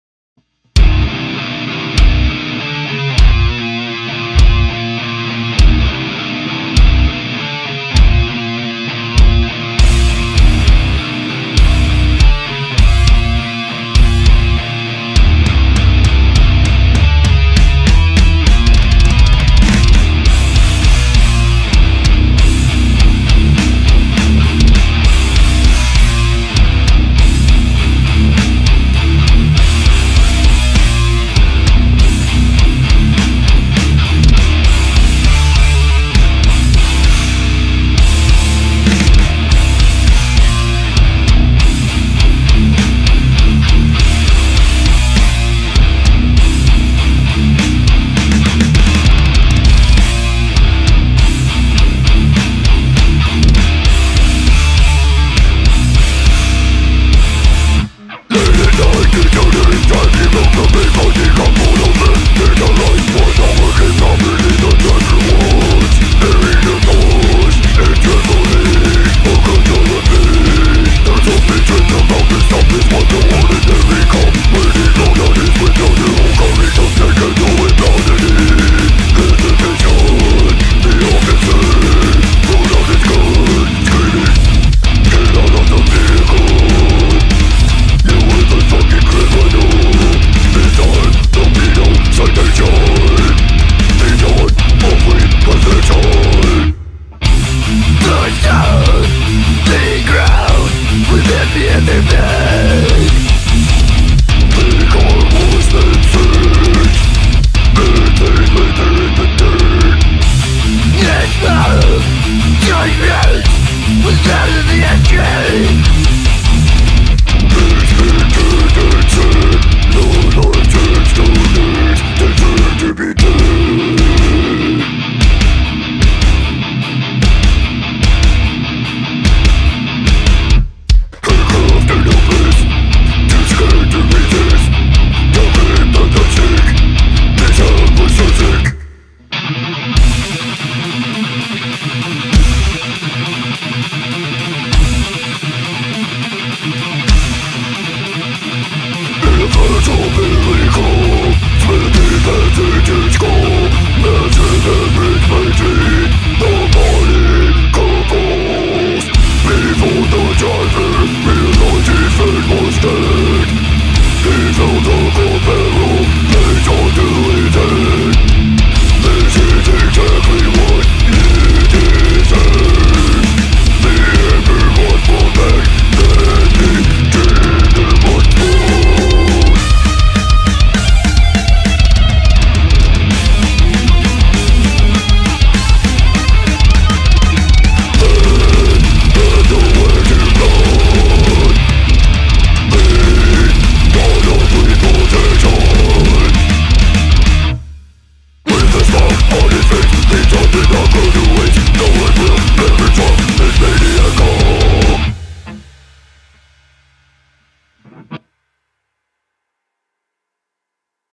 CANNABIS CORPSE Full Performance on Slay At Home | Metal Injection
CANNABIS CORPSE Full Performance from the Slay At Home Fest in support of Musicares / The Iggy Fund / NAMI / Cancer Research Institute DONATE